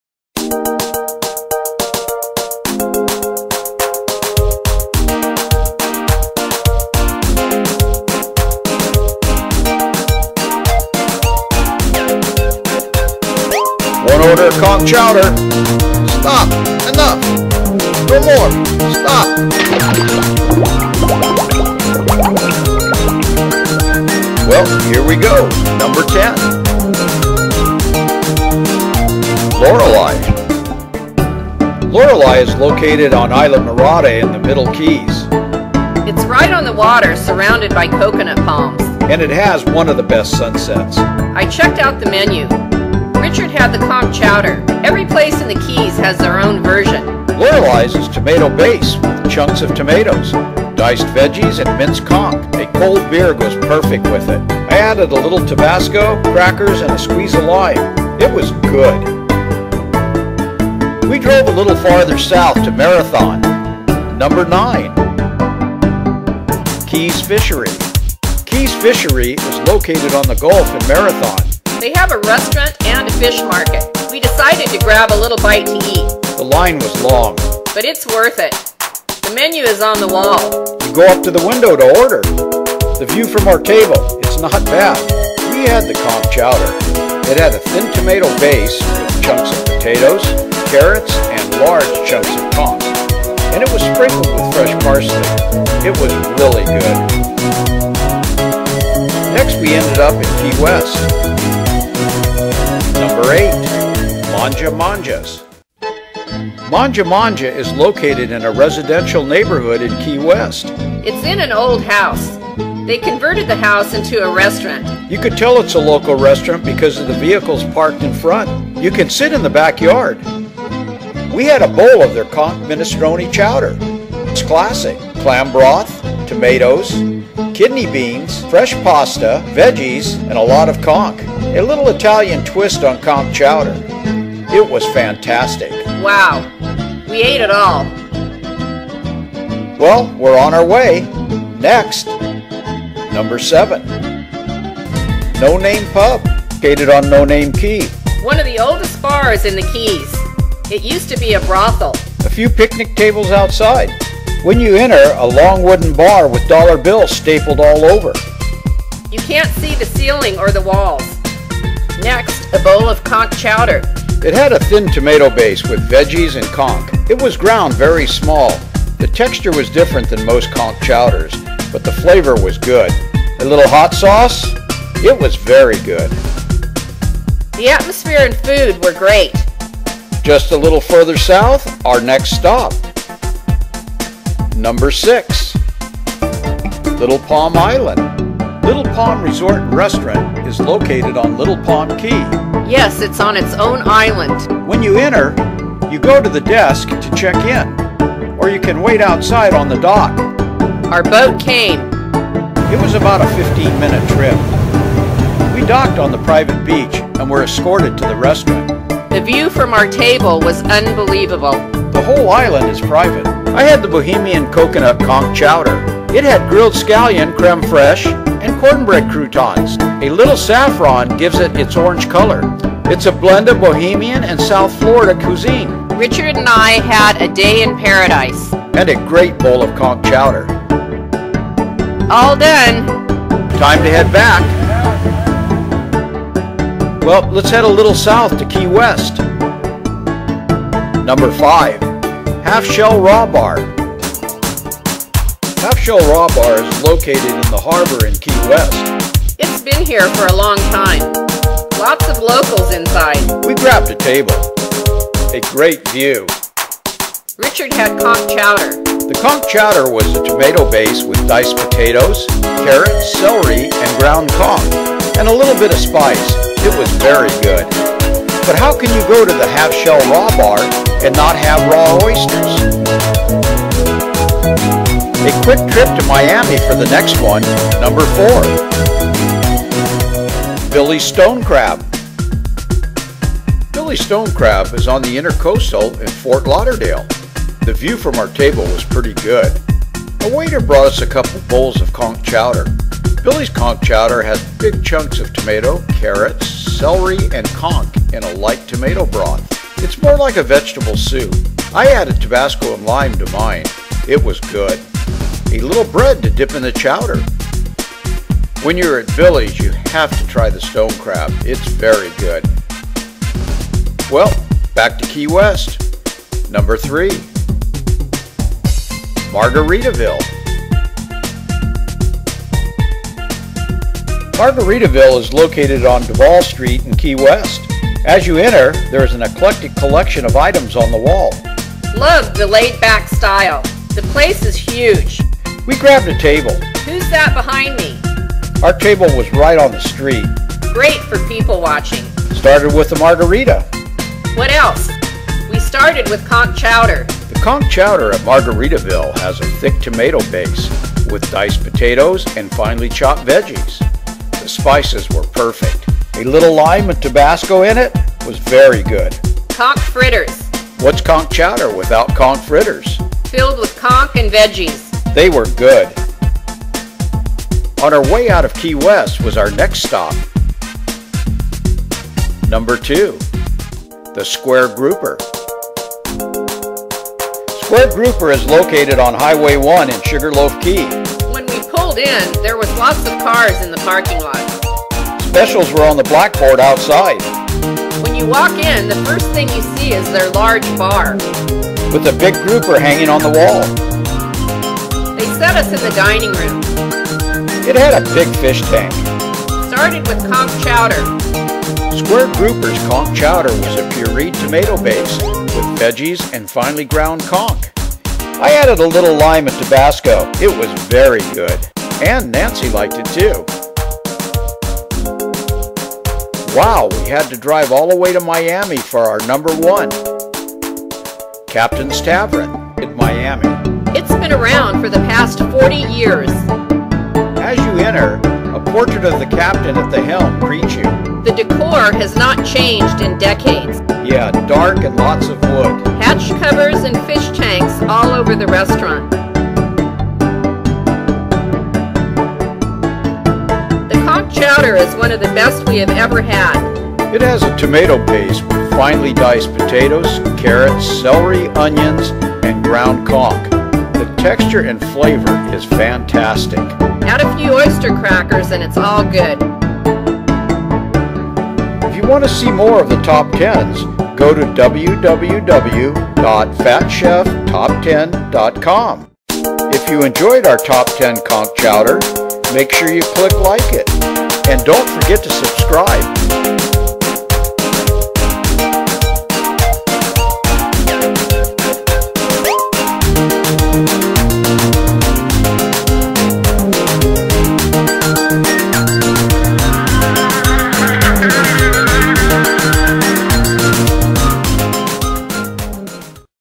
Conch Chowder Top Ten in the Florida Keys and Key West
Richard and Nancy traveled from Miami to Key West, to put together their choices for the Top 10 Conch Chowders. See what they found. If you want to see more go